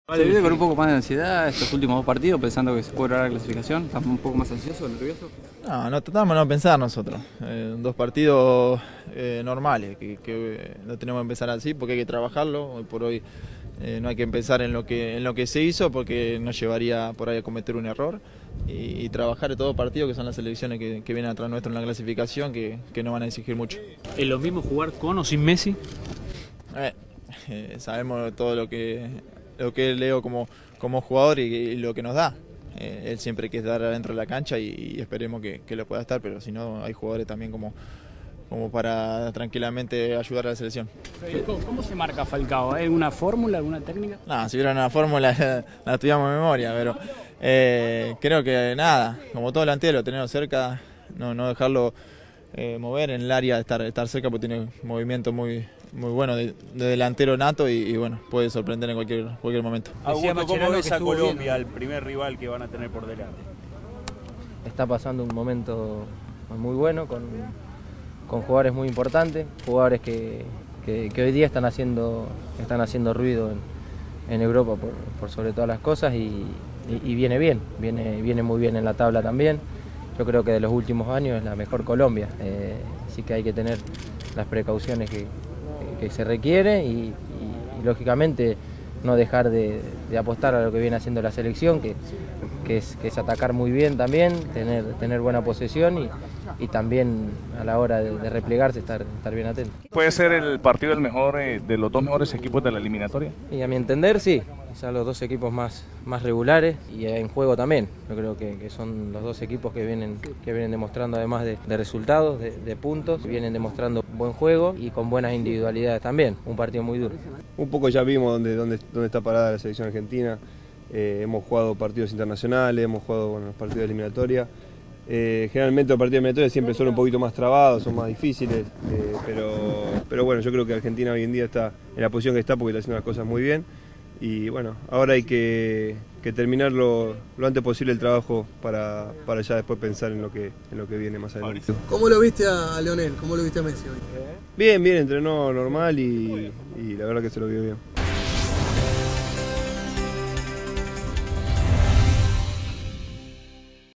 Messi hizo fútbol e ilusiona con su posible presencia
Lionel Messi se entrenó sin problemas y tomó parte de la práctica de fútbol que realizó hoy el seleccionado argentino de fútbol de cara al partido del viernes ante Colombia, por la 13ra. jornada de las eliminatorias sudamericanas para el Mundial Brasil 2014.
La `Pulga`, que se sumó el lunes a la concentración luego de pasar unos días de descanso en Rosario, realizó esta mañana su primer sesión de trabajo y se mostró en óptimas condiciones de la lesión en el bíceps femoral de la pierna derecha que sufrió durante los cuartos de final de la Liga de Campeones de Europa.
El equipo conducido por Alejandro Sabella, líder de la carrera mundialista con 24 puntos, se entrenó esta mañana en el predio de la AFA en Ezeiza para los partidos ante Colombia (tercera con 19) y el martes 11, en Quito, ante Ecuador (segunda con 20).
Sabella dispuso un trabajo táctico en el cual paró al posible once titular para recibir en el Monumental, desde las 19.05, al equipo que conduce el argentino José Pekerman.
La formación, con clásico 4-3-3, fue con Sergio Romero; Pablo Zabaleta, Federico Fernández, Ezequiel Garay y Marcos Rojo; Lucas Biglia, Javier Mascherano y Angel Di María; Lionel Messi, Gonzalo Higuaín y Sergio Agüero.
Biglia, quien parece ganarle la pulseada a Augusto Fernández, se paró sobre el costado derecho del mediocampo y se perfila para ser el reemplazante de José Sosa, quien fue desafectado por un desgarro.
"No es lo mismo jugar con o sin Messi. Es el mejor del mundo y tenerlo es una ventaja. No sé si jugará, pero en el trabajo de hoy lo noté sin ningún tipo de problema", confió su compañero en Barcelona, de España, Mascherano, en medio de una convulsionado atención a la prensa post entrenamiento.
Por su parte, Zabaleta especuló con la presencia del delantero y dijo: "Creemos que va a jugar. Es muy importante para nosotros. El equipo gira en torno a él y tenemos la suerte de que el mejor del mundo sea argentino".
"Estamos confiados en hacer un buen partido. Por lo que ví hoy vamos a poder contar con Lio", comentó Fernández, al tiempo que resaltó: "Es un jugador clave para nosotros. El equipo tiene más jerarquía con él y no estamos pensando en que no juegue".
Por otra parte, los mediocampistas Fernando Gago y Eber Banegas y los defensores Gino Peruzzi y Hugo Campagnaro se entrenaron diferenciados por diferentes problemas físicos.
El plantel `albiceleste` volverá a trabajar mañana desde las 10.30, a puertas cerradas, y luego los jugadores quedarán libres hasta el jueves, día en volverán a trabajar y que el entrenador nacional, Sabella, dará una conferencia de prensa desde las 11.45.
Argentina será local el viernes, desde las 19.05, ante Colombia y el domingo, a las 17, viajará rumbo a la ciudad de Guayaquil para luego, horas antes del partido, viajar a Quito, donde se medirá con Ecuador en el estadio Olímpico Atahualpa.